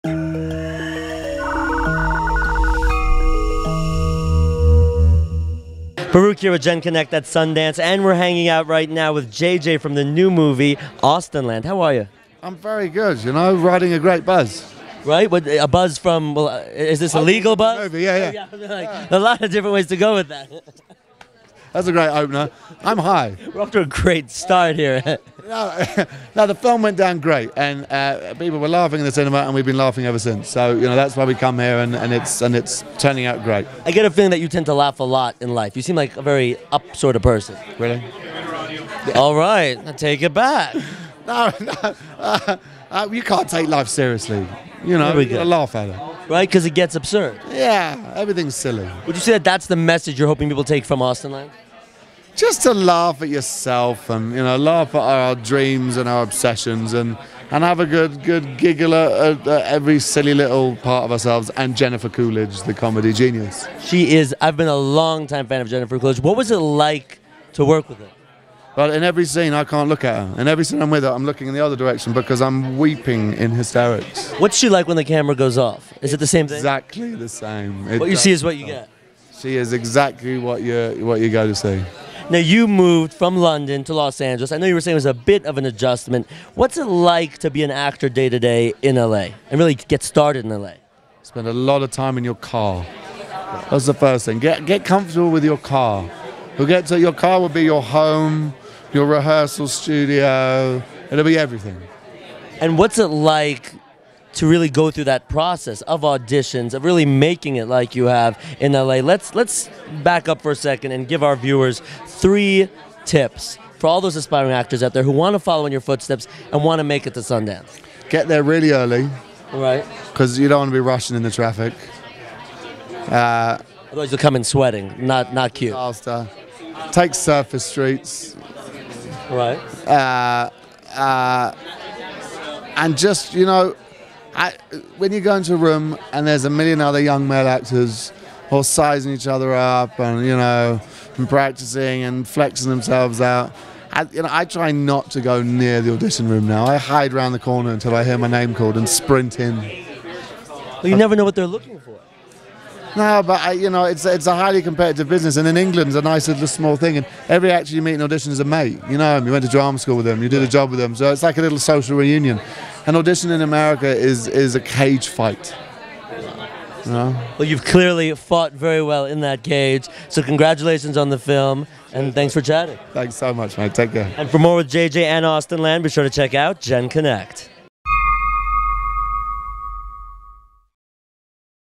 Actor JJ Feild On Role in Sundance Film 'Austenland' | genConnect
JJ Feild discusses his role in the film Austenland at the Sundance Film Festival